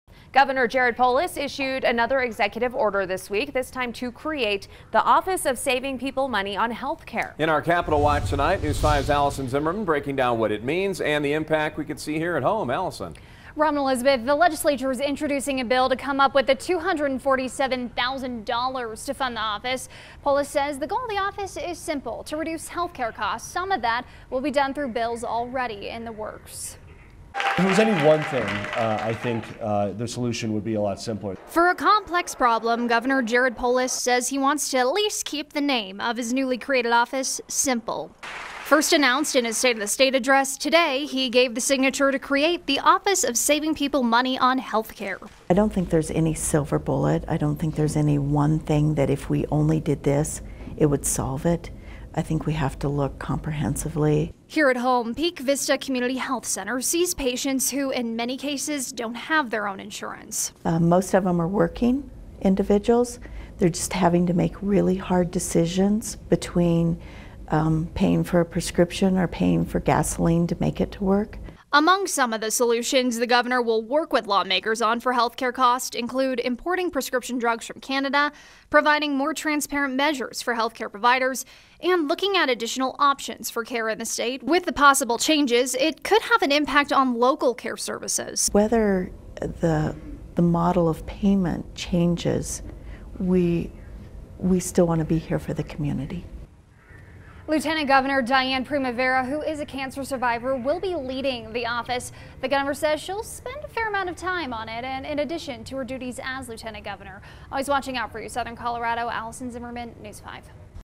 Gov. Polis creates office to examine lowering health care costs
'The Office of Saving People Money on Health Care' is now in the works, as Governor Jared Polis signed an executive order to create the office.